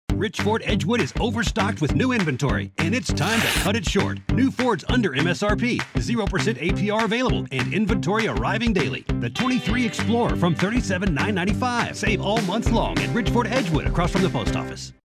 Rich Ford Edgewood 2023 Short Sale #4
Rich Ford Edgewood is overstocked with new inventory, and it’s time to cut it short!
New Fords under MSRP, 0% APR available, and more inventory arriving daily!
Get the 2023 Ford Explorer from $37,995.
Save all month long at Rich Ford Edgewood, across from the Post Office.